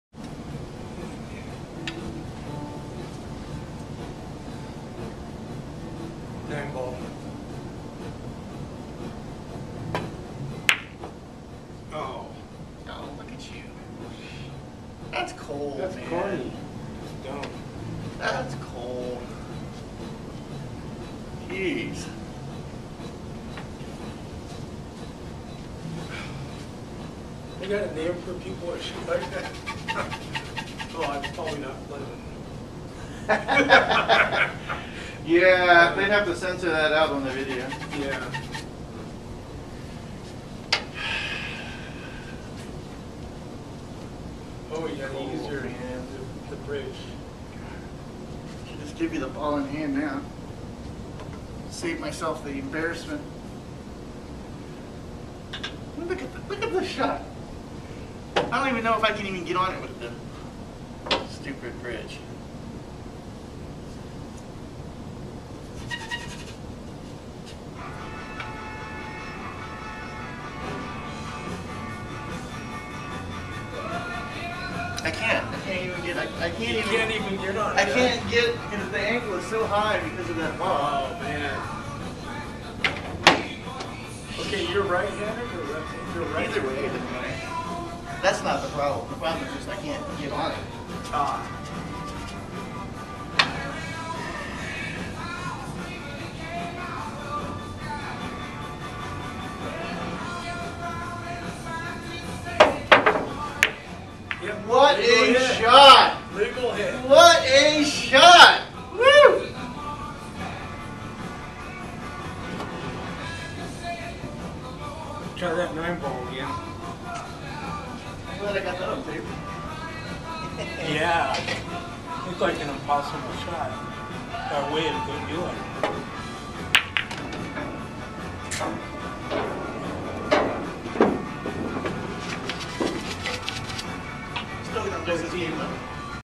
Hooked & Escape
Alaska Challenge 8-Ball Tournament at Beluga Billiards. Match between Ted Rado and Sean Weems. Ted hooks Sean hard, but after much consternation, Sean escapes with a miraculous jump shot off the rail.